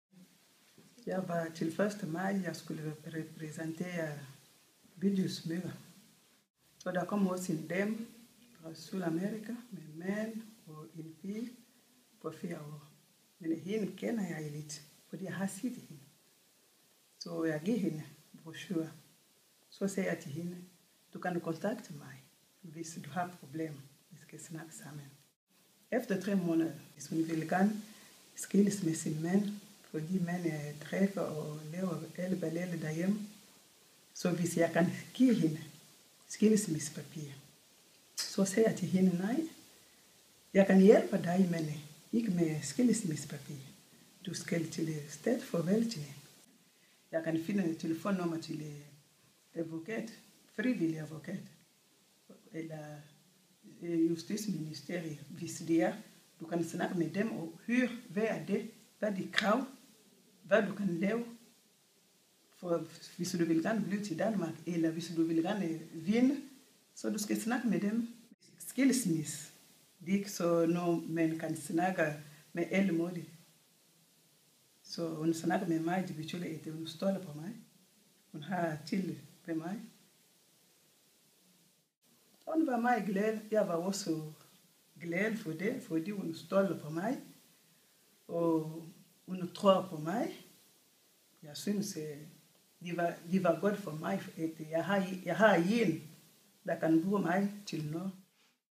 Georgettes Forandringshistorie
I filmen fortæller Georgette en forandringshistorie fra sit arbejde som Bydelsmor.